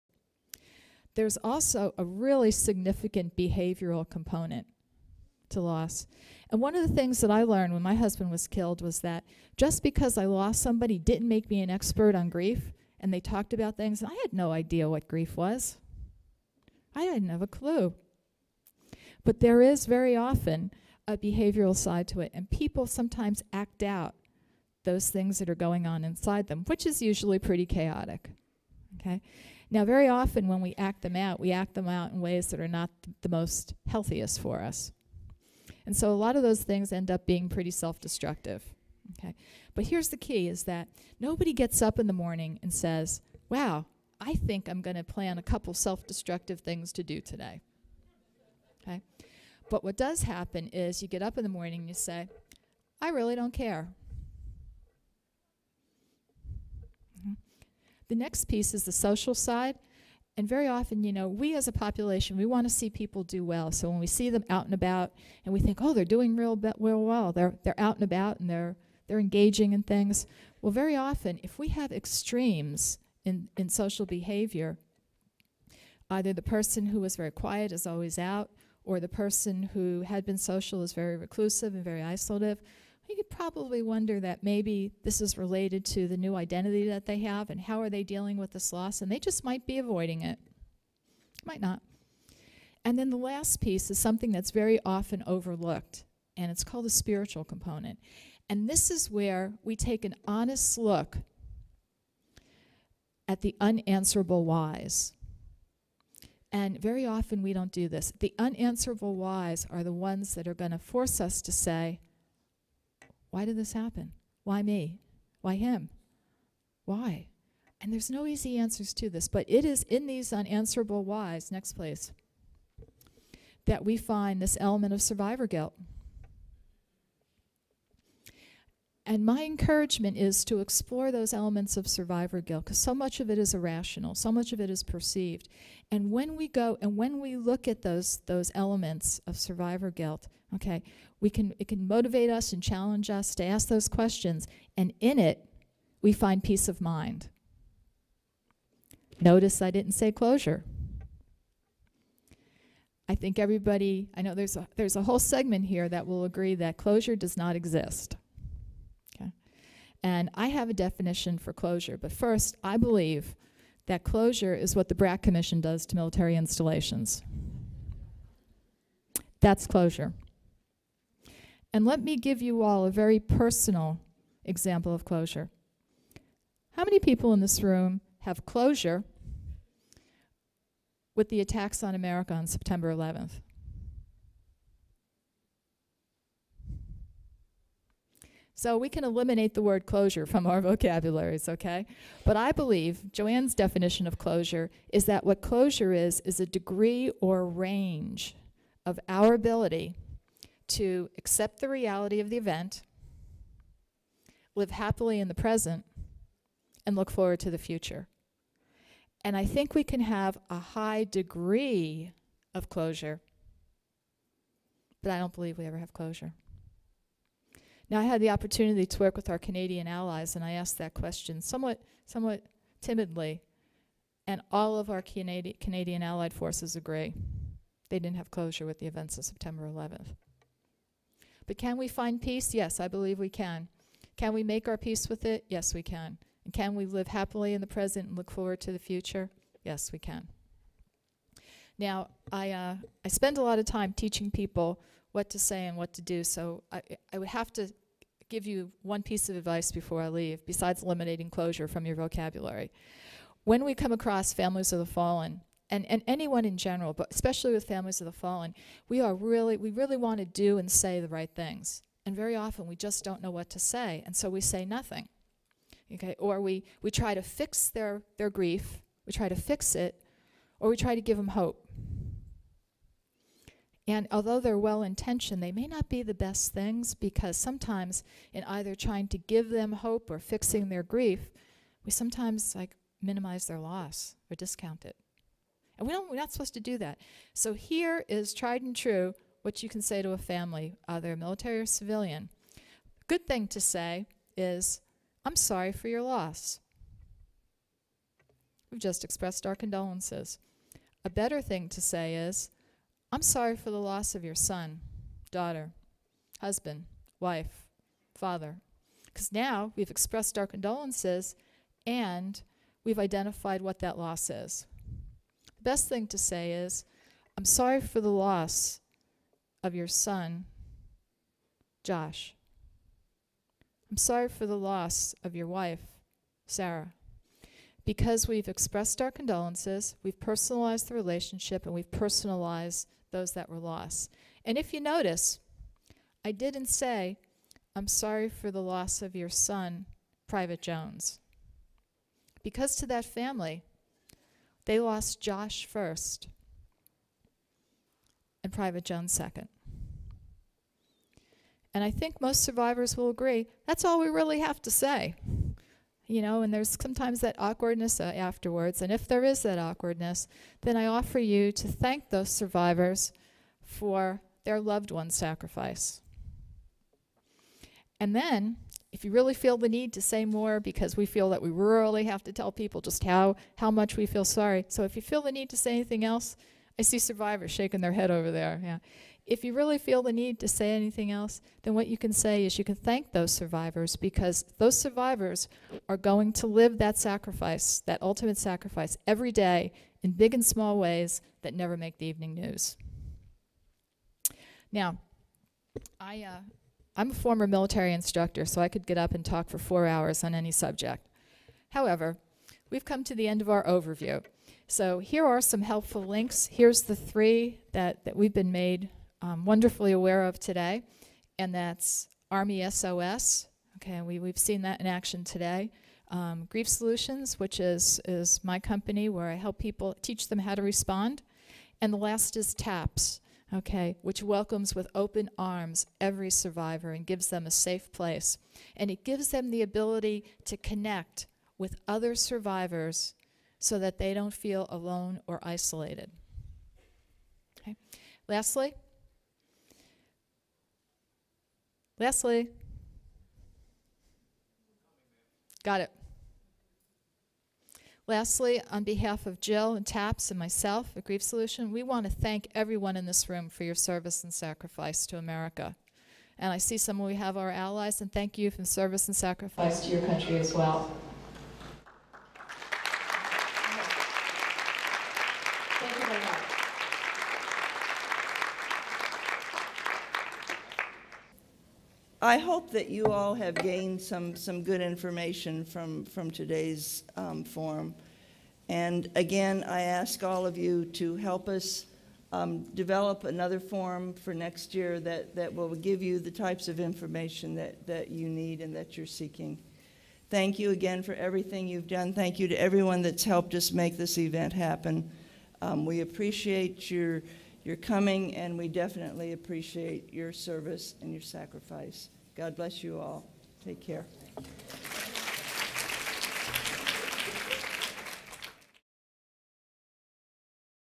2010 AUSA Family Forum IV, part 5
Col. Wendy Martinson moderates the fourth Family Forum at the 2010 AUSA Annual Meeting & Expo, titled, "America's Families - Caring for Our Survivors." Part 5 of 5.